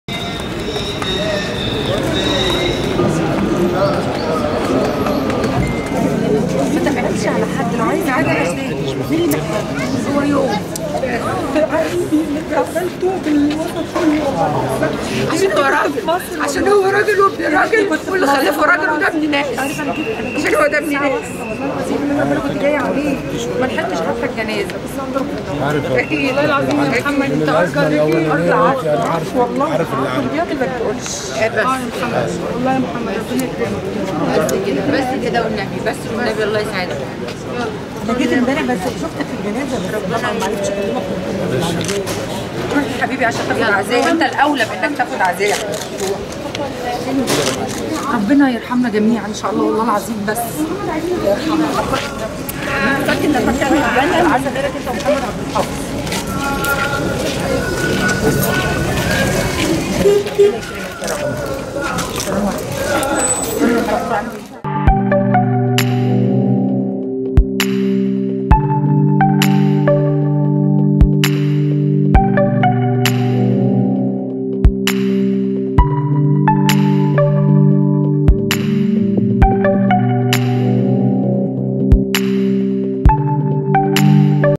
وفاء عامر لـمحمد عبدالحافظ في عزاء ماهر عصام| راجل ابن راجل
وفاء عامر لـ"عبدالحافظ" في عزاء ماهر عصام: "راجل ابن راجل"
تقرير: محمد فاروق

جريدة الوطن
جريدة يومية مستقلة